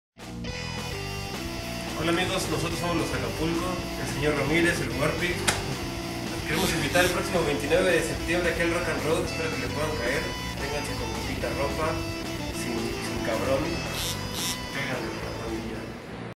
Lost Acapulco en el Rock & Road 29-sep
Lost Acapulco inundará el escenario del Rock and Road con sus camisetas de playa, bermudas y máscaras, con el concierto que darán en ese lugar el próximo sábado 29 de septiembre en las instalaciones del Rock and Road.